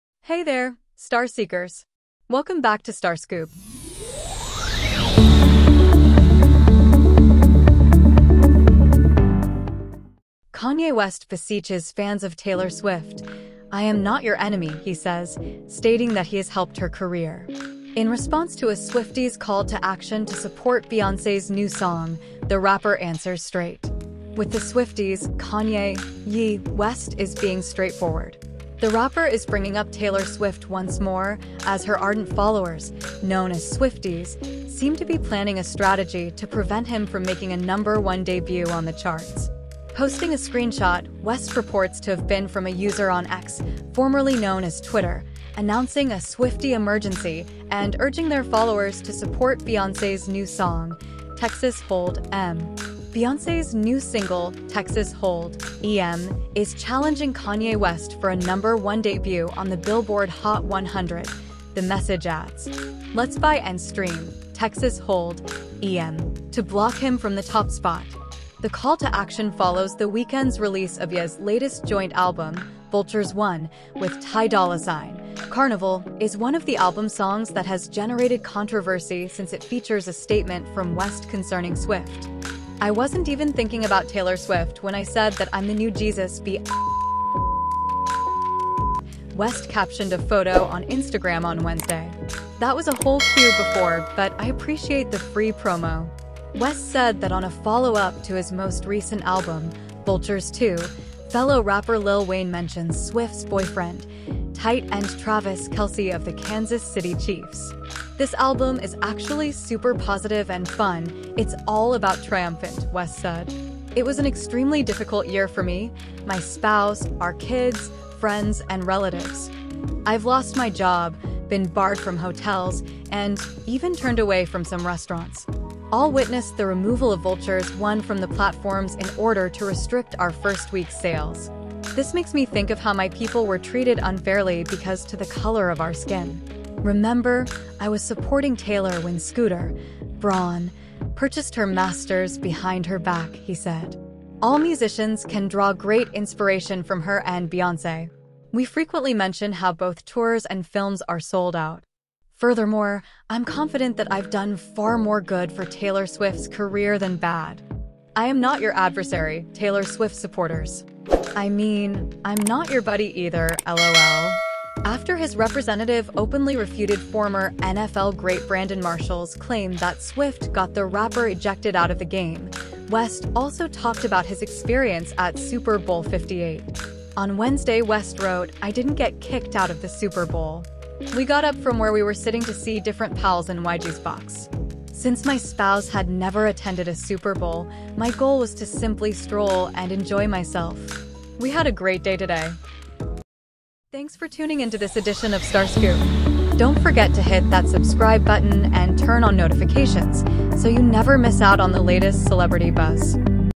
Kanye West Addresses Taylor Swift Fans: 'I Am Not Your Enemy'
Kanye West breaks silence, insisting he's not an adversary to Taylor Swift fans, citing his contributions to her career. Dive into the latest celebrity feud developments and join the conversation!

song description
No Podcast
No Song Making
No Audiobook